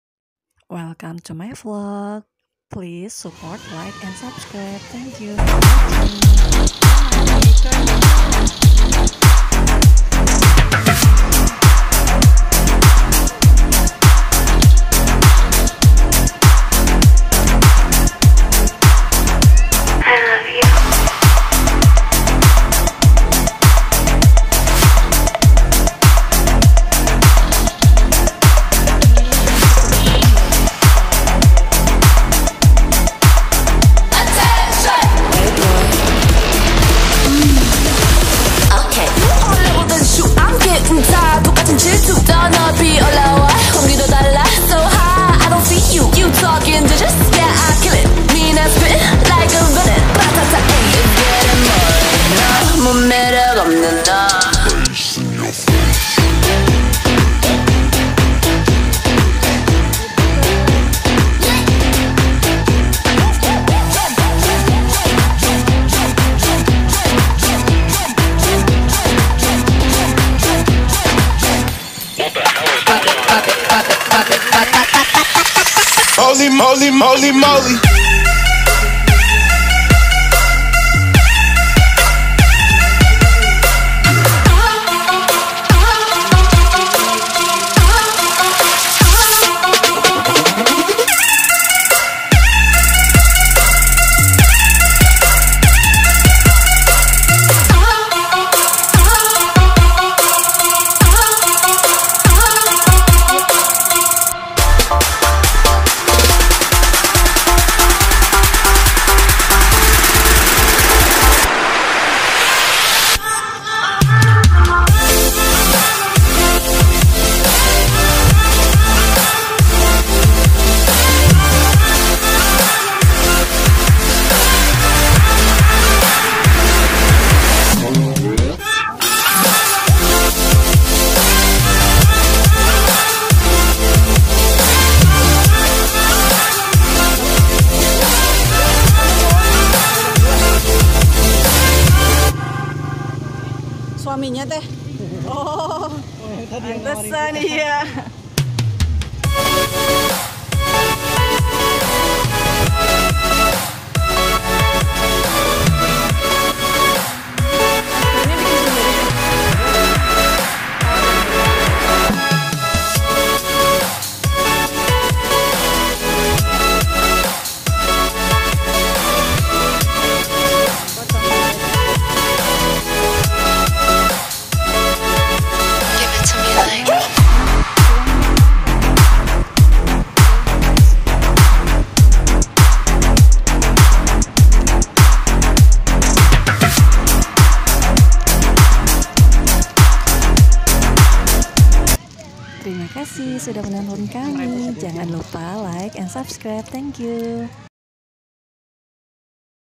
Bawa Pulang Ondel- Ondel Situ Babakan Srengseng Sawah
Part 2 Situ Babakan Srengseng Sawah Depok

Disini wisata keluarga murah tepatnya diperkampungan Betawi yang berada di Srengseng Sawah depok, Akses mudah dan Sangat Rekomendasi untuk dikunjungi Harga tiket masuknya murah dan banyak permainan Anak dan Makanan² yg enak